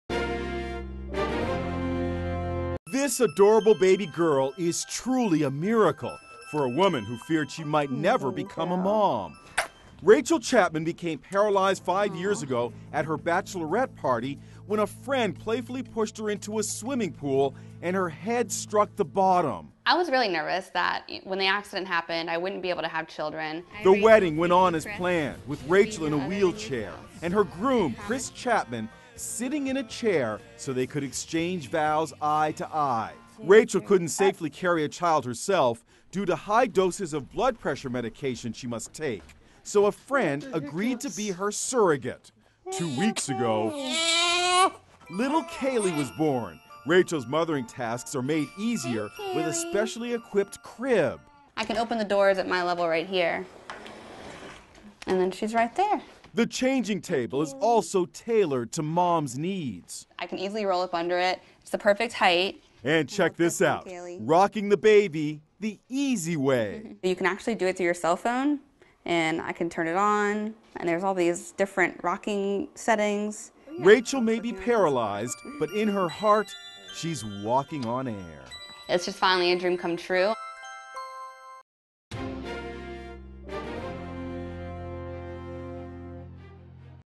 'Paralyzed Bride' Celebrates Mother's Day with New Baby
Rachelle Friedman Chapman was paralyzed from the chest down following an accident at her bachelorette party. INSIDE EDITION visited her home and she showed us how she handles motherhood.